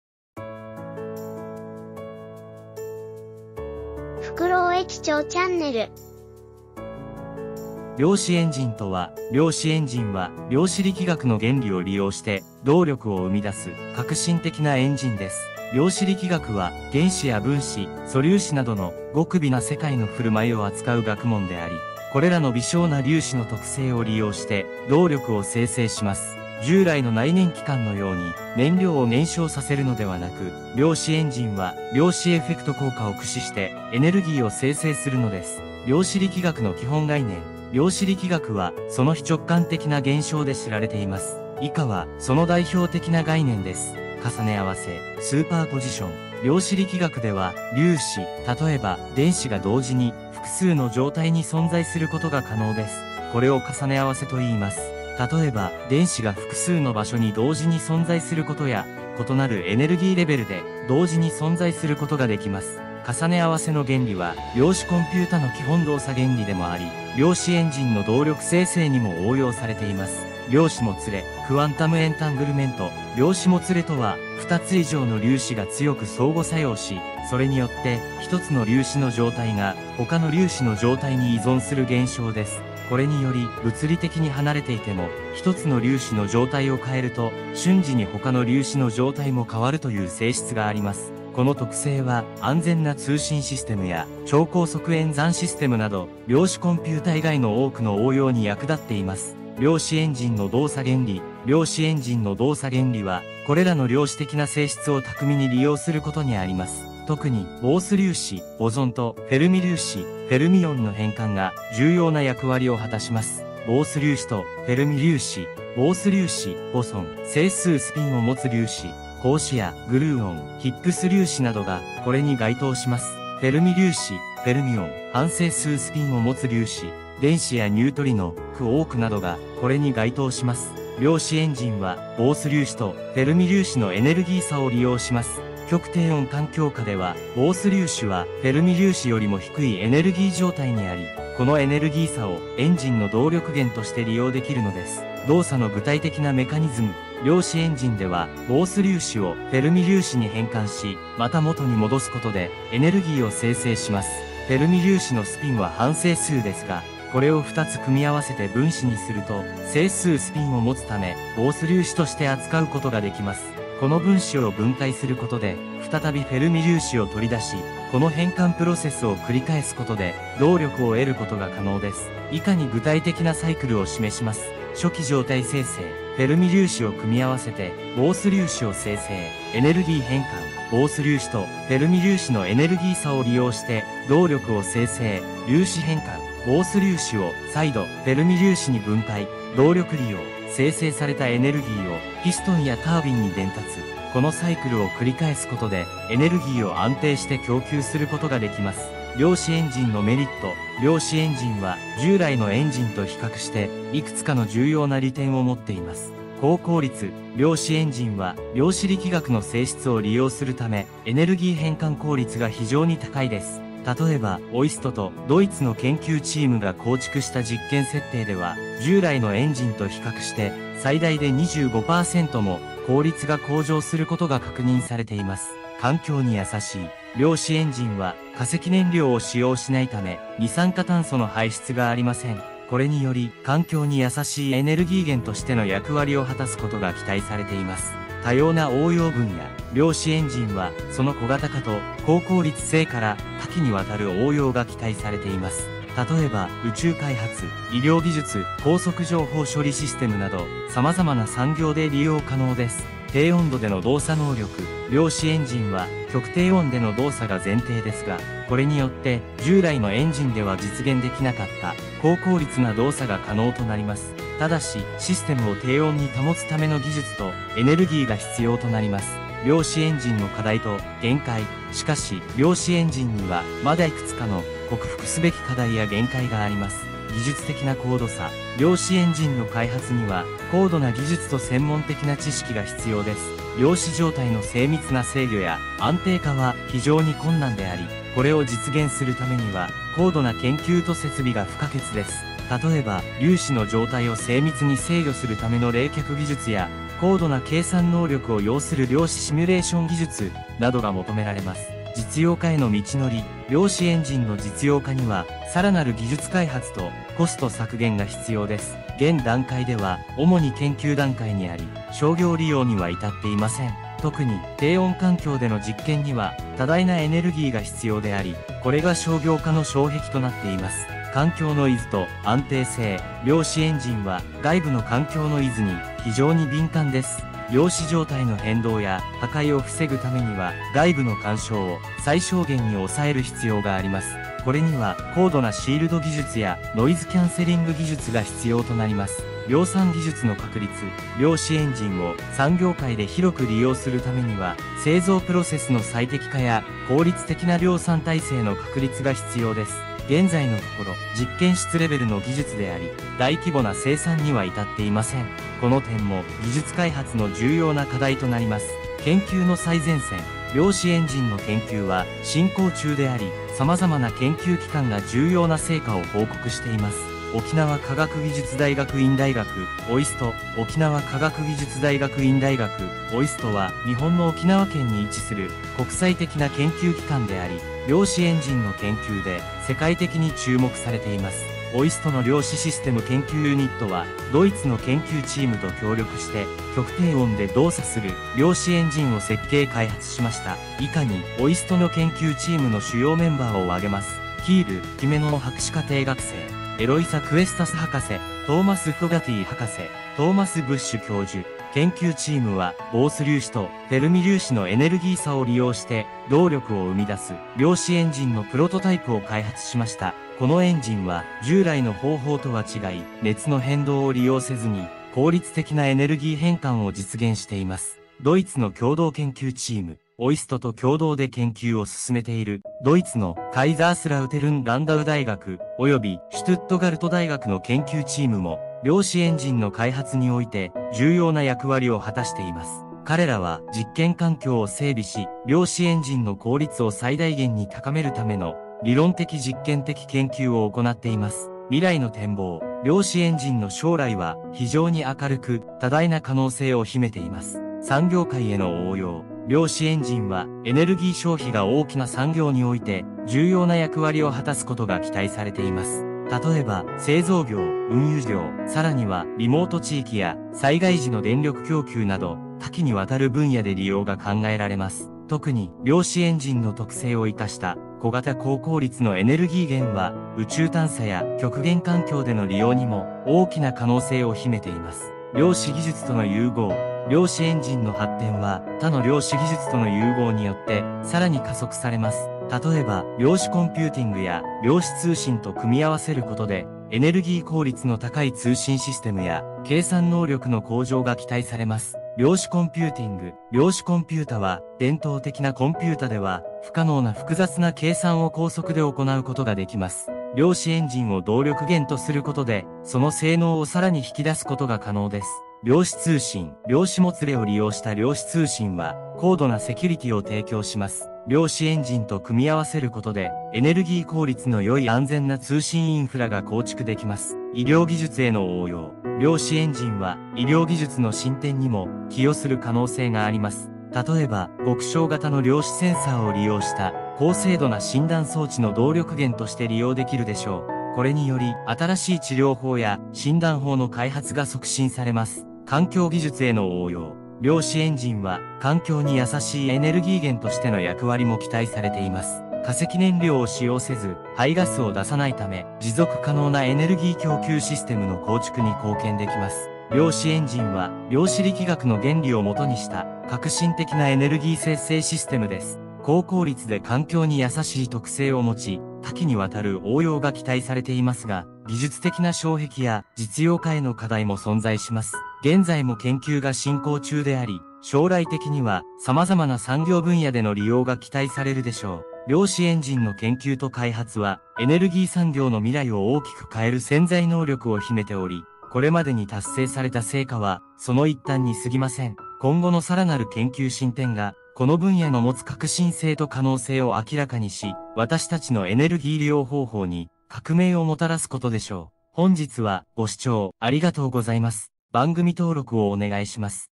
沖縄から世界へ！量子エンジンの秘密：未来のエネルギー革命がここに！
量子エンジンとは、量子力学の原理を利用して動力を生み出す画期的な技術です。この動画では、量子エンジンの基本原理から、その多様な応用可能性について分かりやすく解説します。高度な量子技術によって、高効率かつ環境に優しいエネルギー生成が可能となり、将来的には様々な産業分野での利用が期待されています。OIST（沖縄科学技術大学院大学）とドイツの研究チームによる最新の研究成果を紹介し、量子エンジンが私たちのエネルギー利用方法にどのような革命をもたらすのかを見ていきましょう。未来のエネルギーが気になる方、技術革新に興味がある方は必見です！

参考文献
量子革命の原動力、「量子エンジン」が実現する日も近い？
量子エンジンとは？実用化の可能性や課題、最新の研究動向を解説
未来のエネルギーはここから: 量子エンジンの基本と可能性